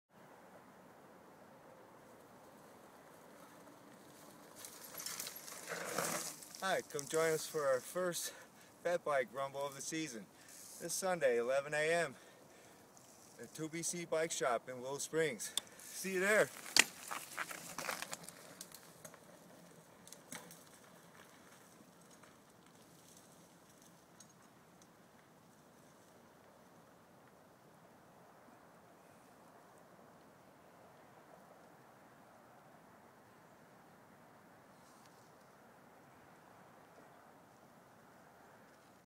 2 Bici Presents: A Message From Rick
Rick invites anyone and everyone with a fatbike to come out and explore some of his favorite trails in and around the Palos Forest Preserve.